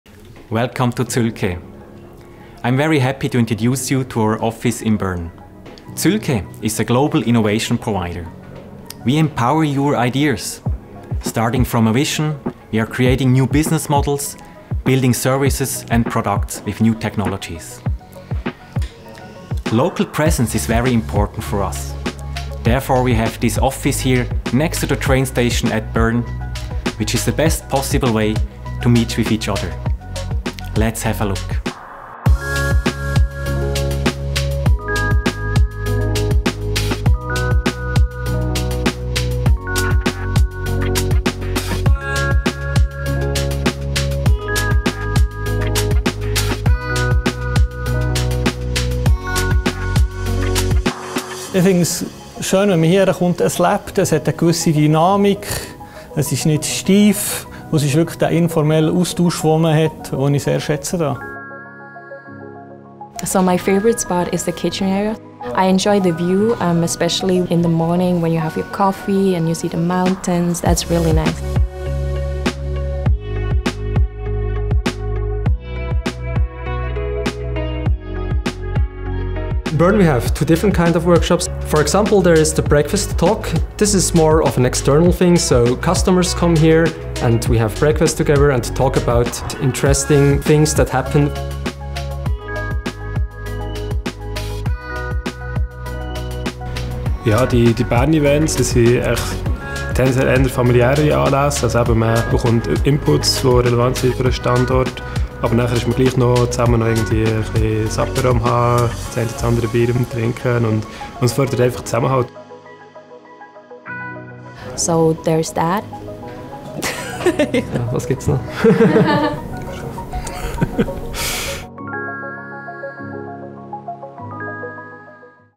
Zühlke Switzerland - A glimpse into our Bern office
At Zühlke you are where innovation happens. Our office in Bern is conveniently located next to the main train station, on the sixth floor of the PostParc building. Take in the stunning view of the Swiss Alps while you're here. Come visit us and see for yourself!